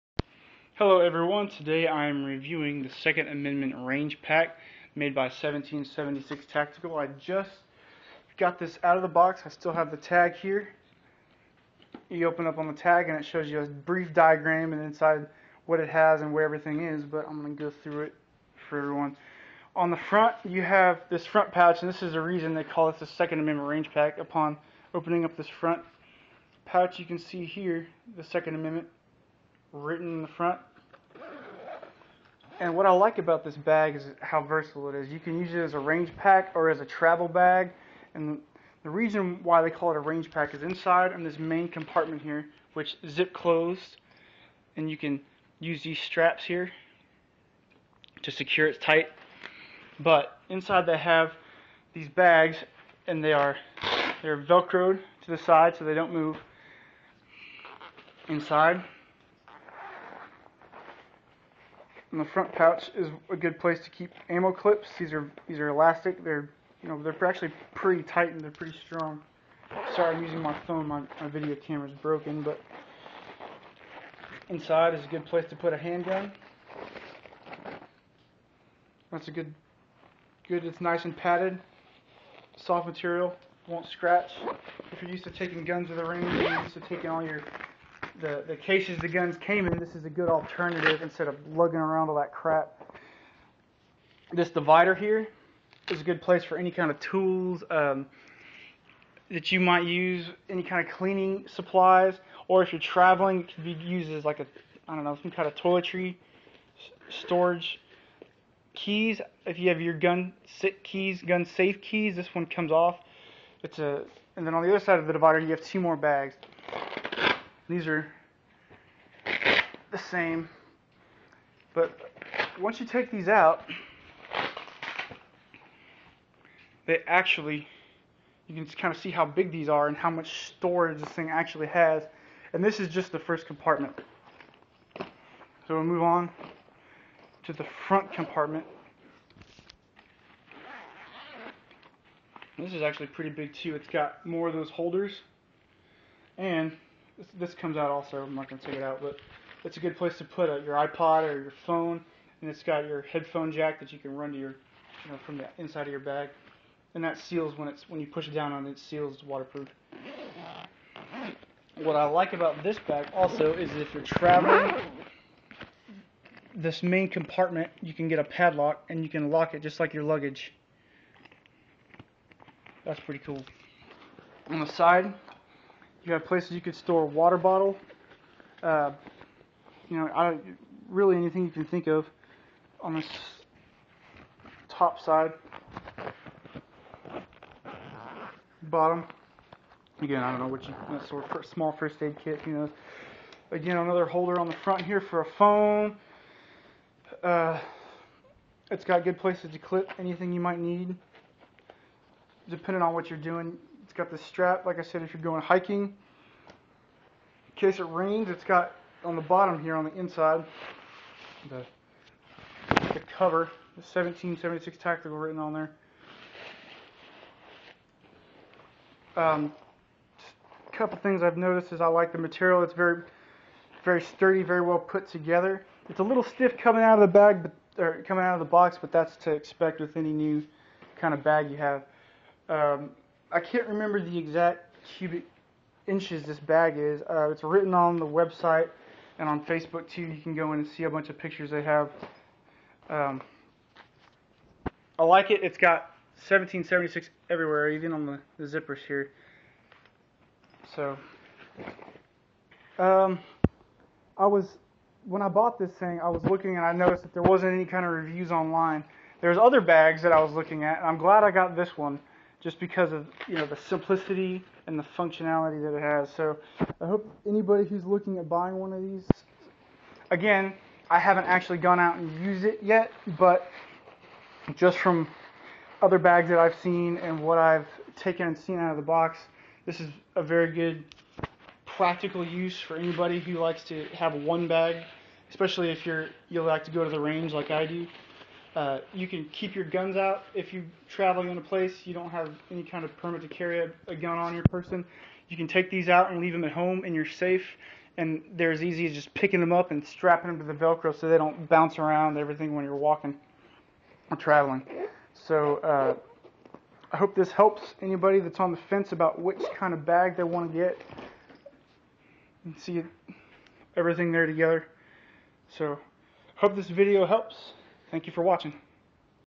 Second Amendment Range Pack Review
The Second Amendment Range Pack by 1776 Tactical Review. The bag is right out of the box.